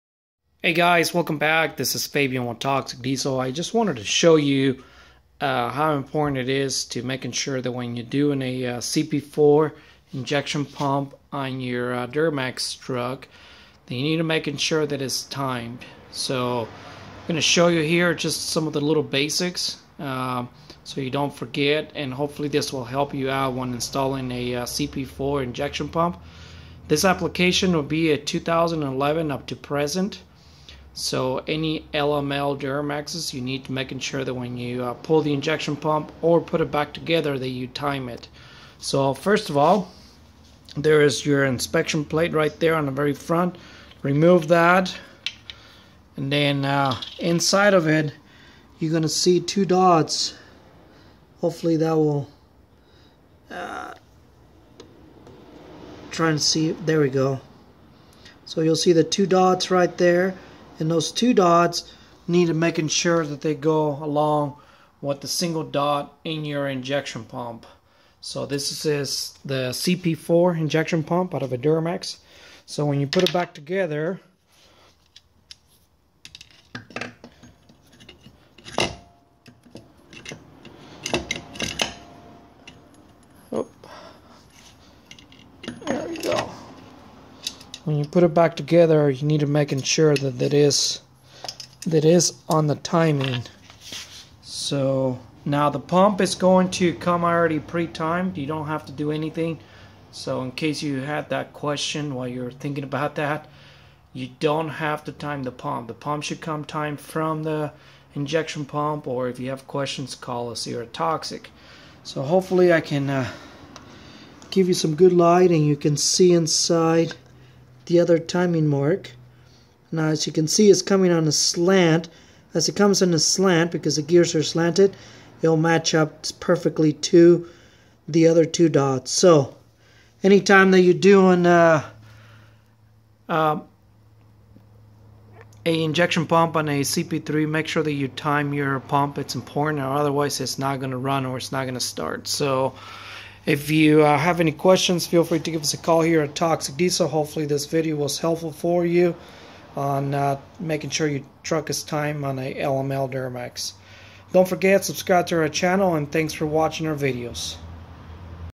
LML Duramax Cp4 Pump Timing
How to time your LML Duramax Cp4 fuel injection pump.